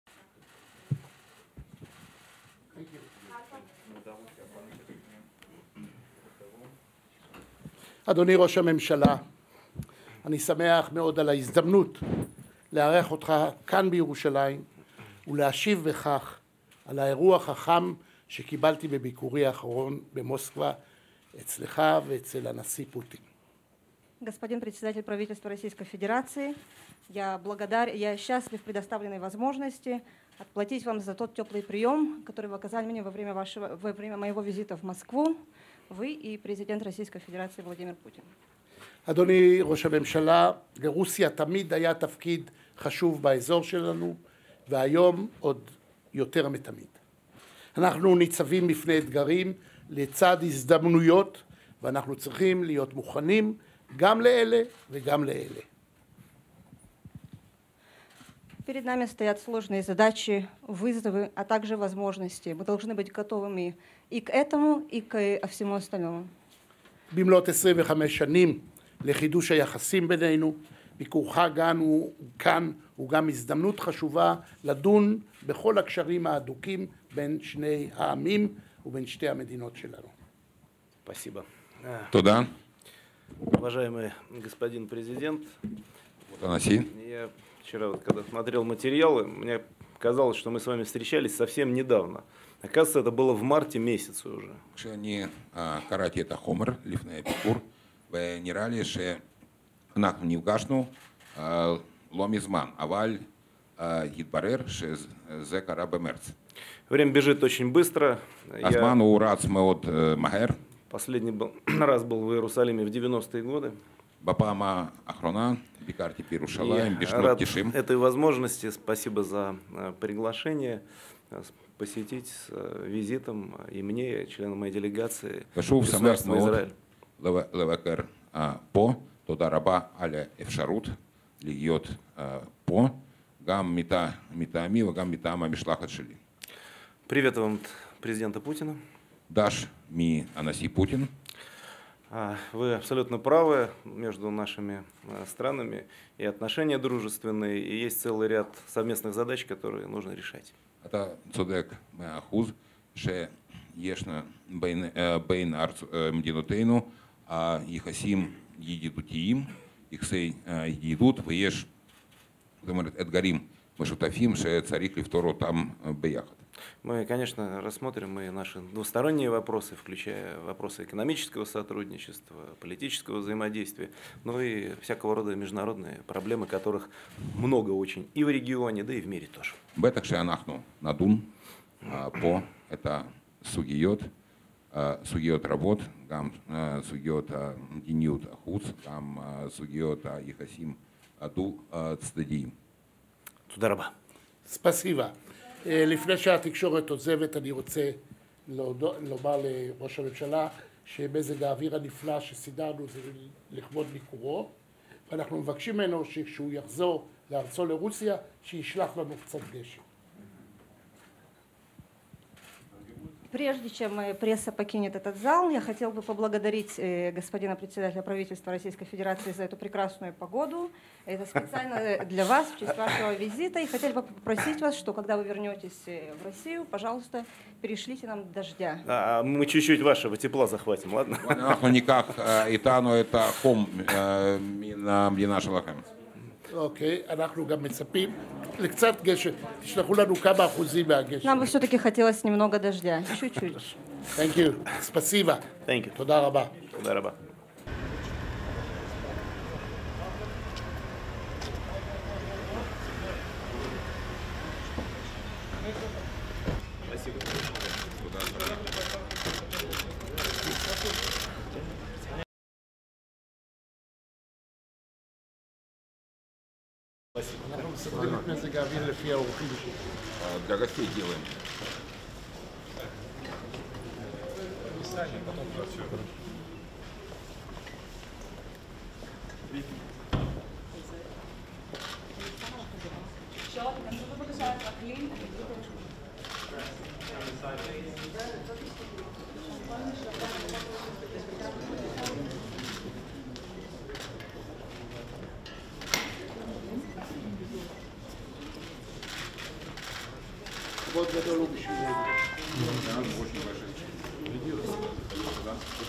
President Rivlin welcomes Prime Minister Medvedev of Russia
President Reuven Rivlin this morning (Thursday) held a working meeting at his residence with Russian Prime Minister Dmitry Medvedev. Before the meeting both men gave brief public statements.

President Rivlin welcomed the Prime Minister and said, “Mr. Prime Minister, I am happy for the opportunity to welcome you here in Jerusalem, and to repay the warm hospitality I received on my visit to Moscow earlier this year.”

He stressed, “Russia has always had an important role in our region, perhaps today more than ever. We are faced by many challenges, and at the same time opportunities, and we need to be prepared for both.”